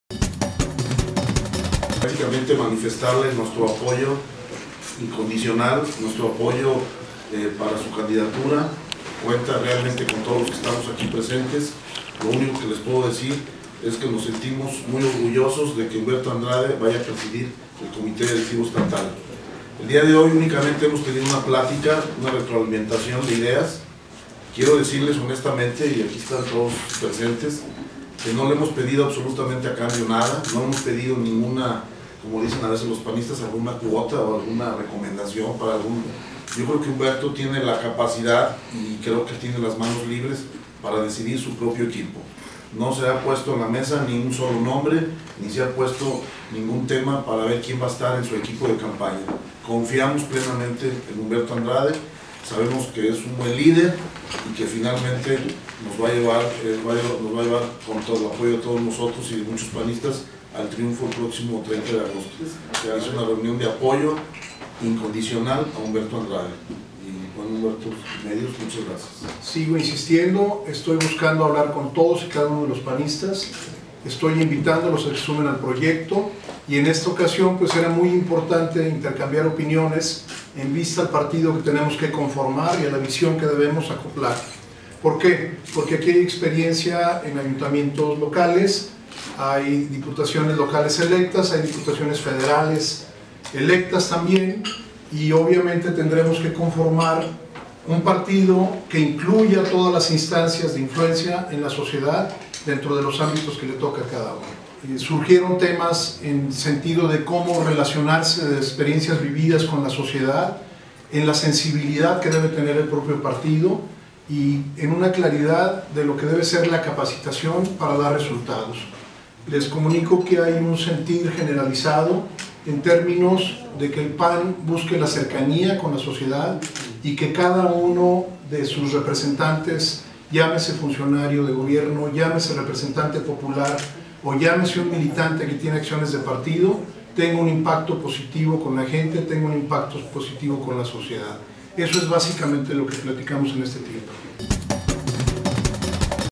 Salim y Sheffield apoyan a Humberto Andrade
Panistas simpatizantes de Miguel Ángel Salim Alle se sentaron ayer por la mañana a desayunar con Ricardo Sheffield Padilla, para apoyar al aspirante a la dirigencia del PAN estatal, Humberto Andrade Quezada.